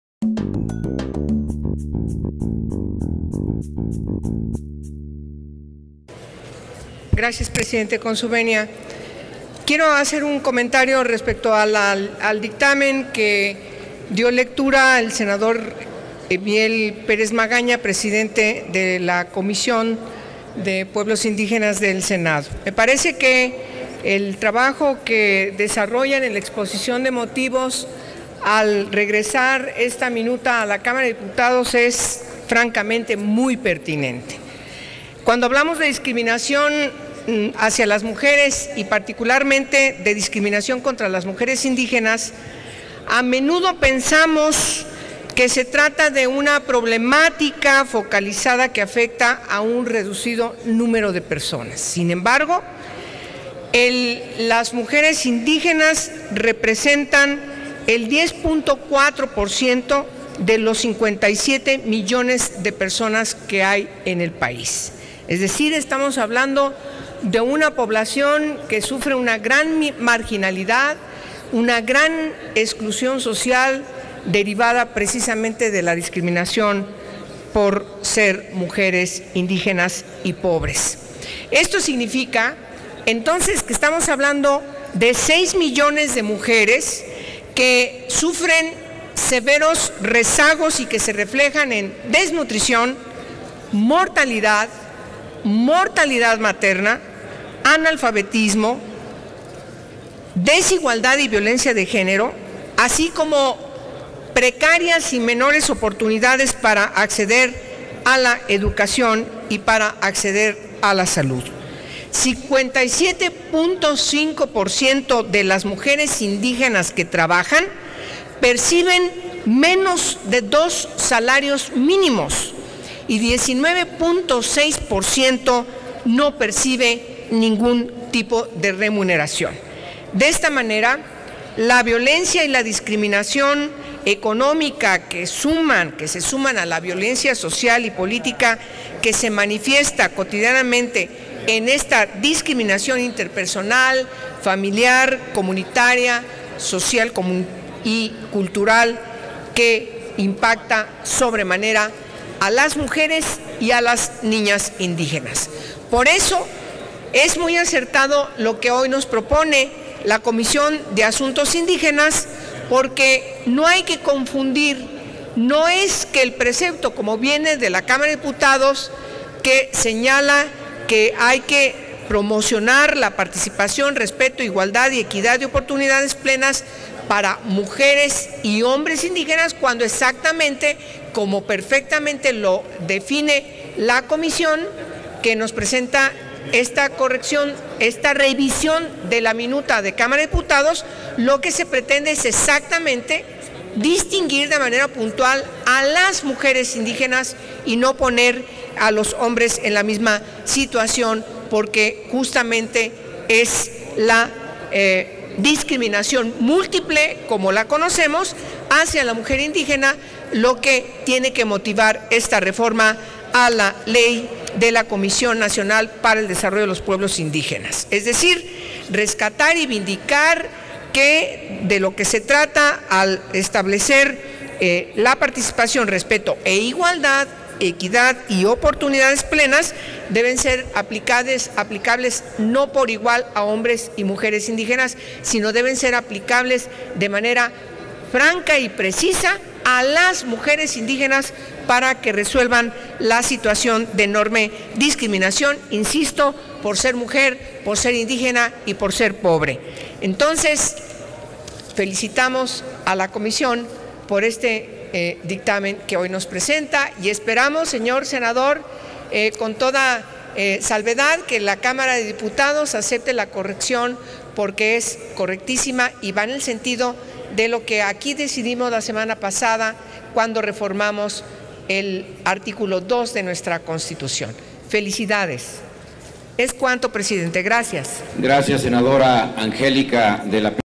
10/12/2014 Sen. Angélica de la Peña - Cuarta intervención en tribuna
Intervención en tribuna de la senadora Angélica de la Peña Gómez para referirse a favor del dictamen por el que se reforma el artículo 3, fracción V, de la Ley de la Comisión Nacional para el Desarrollo de los Pueblos Indígenas.